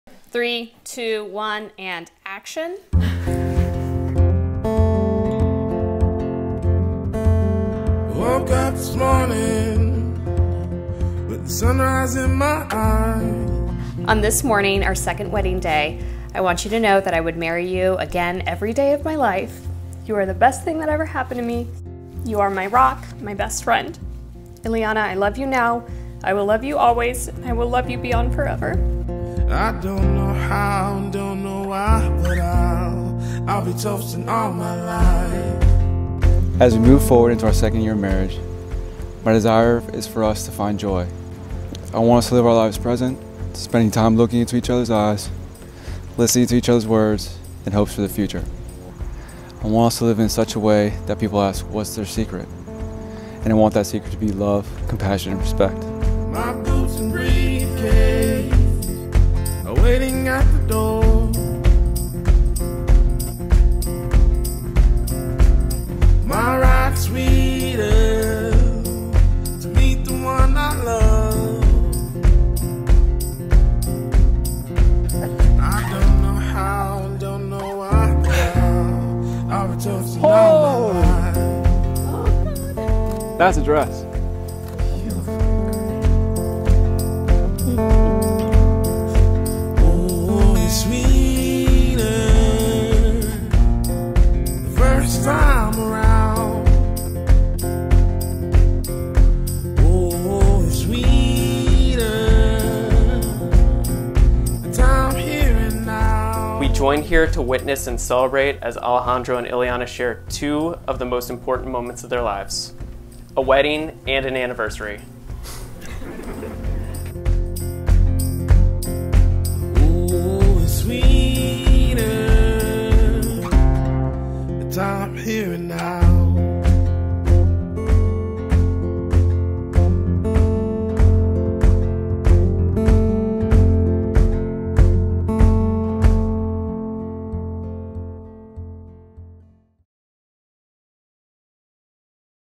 3.. 2.. 1.. and Action (A Brookeville Maryland Wedding Video)
A sneak peek at Illeanna & Alejandro's beautiful wedding day at Bella Vita Farm in Brookeville, MD!